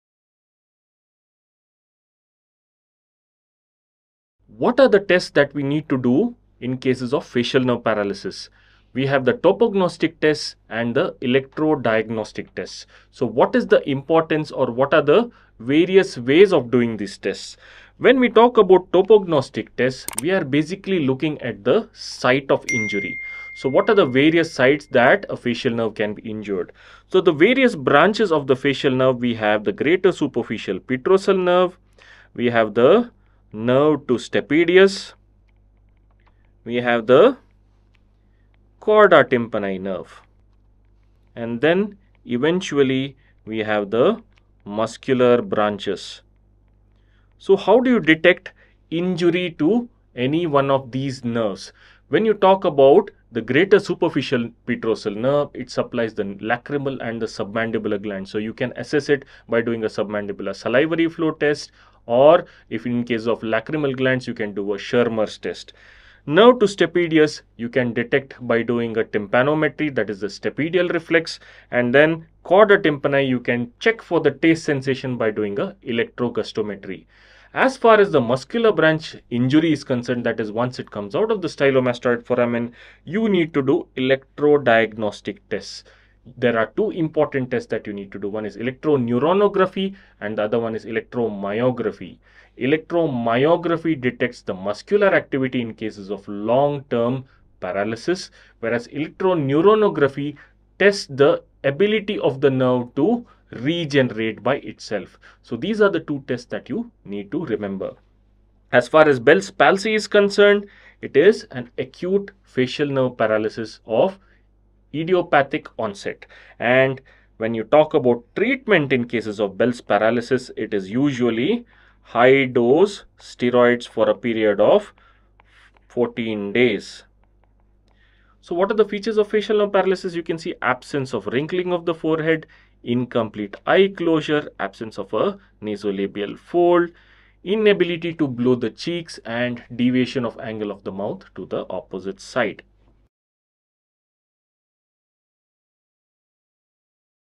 NEET-PG Prep | ENT | Facial Nerve Palsy by Dr. Ajay Bhandarkar
Explore the complexities of Facial Nerve Palsy with Dr. Ajay Bhandarkar. Mastering this ENT topic is essential for NEET-PG 2024 success. Join us now to accelerate your ENT revision and conquer NEET-PG together!